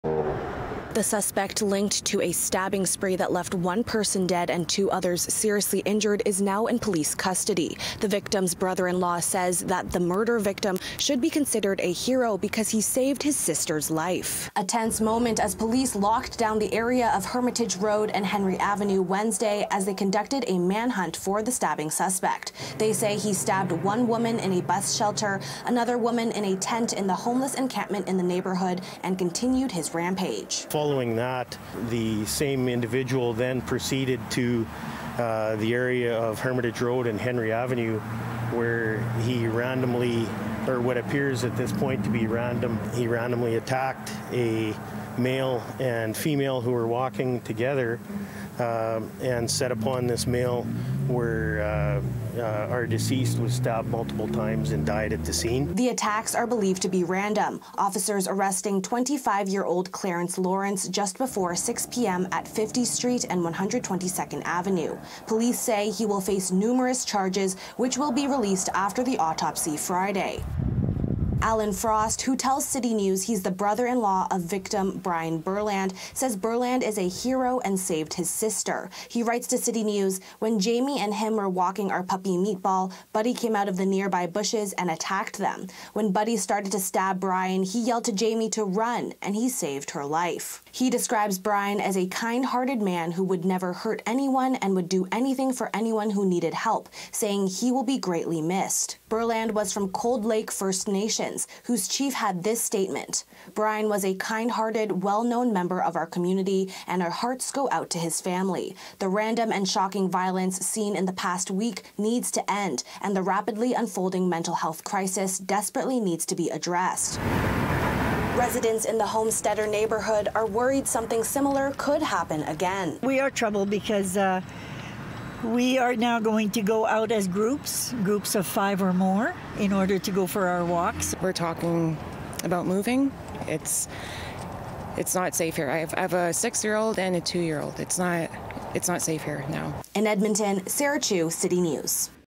Victim of Edmonton stabbing spree called a “hero” who saved his sister’s life
A stabbing spree in Edmonton has left one man dead and two women seriously injured. But Sarah Chew says one victim is being hailed as a hero for saving one life.